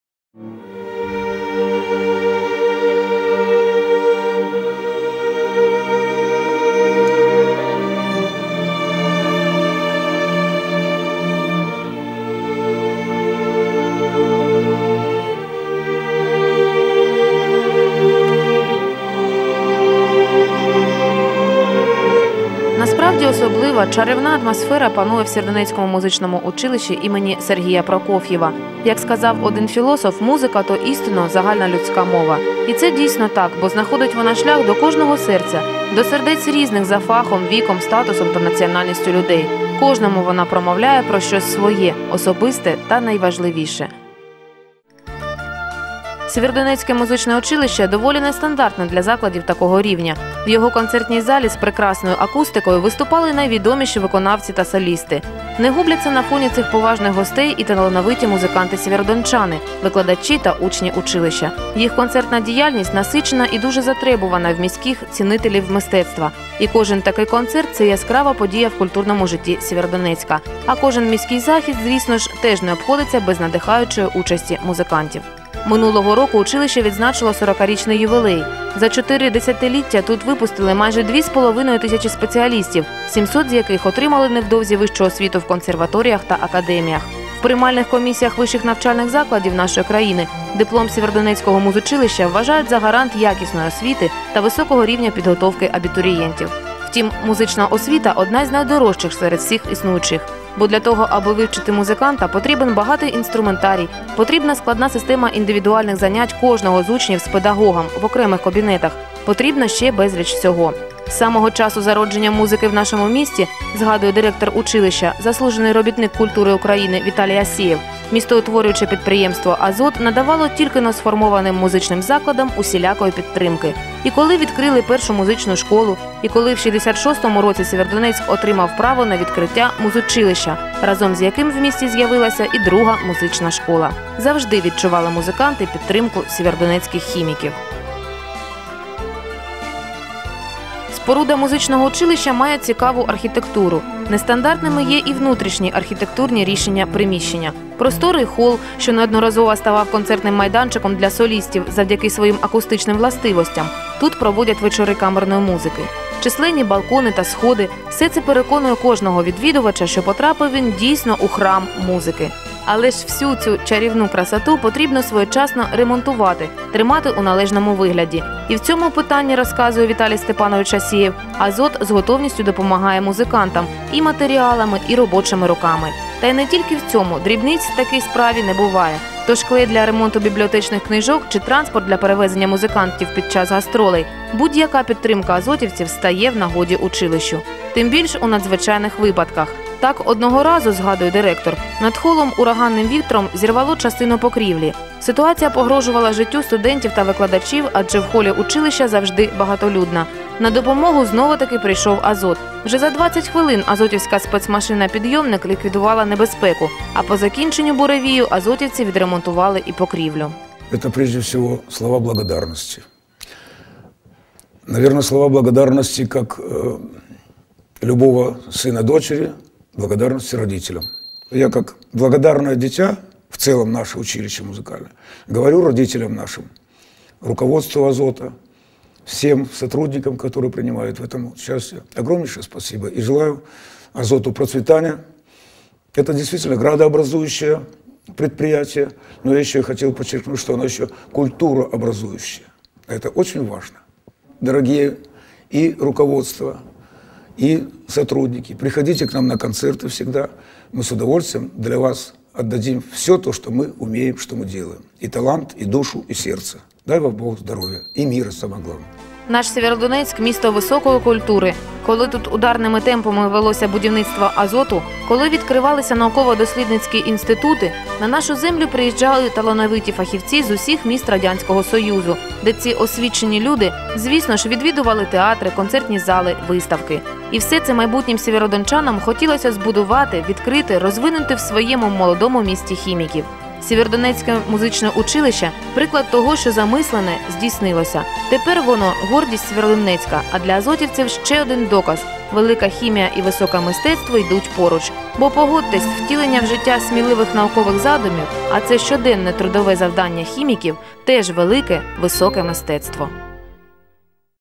З історії Сєвєродонецького коледжу культури і мистецтв. Частина 1
З історії розвитку Сєвєродонецького коледжу культури і мистецтв імені Сергія Прокоф'єва. Частина 1. Автор - Н. Курлат, Камера і монтаж - О. Ізвьосткін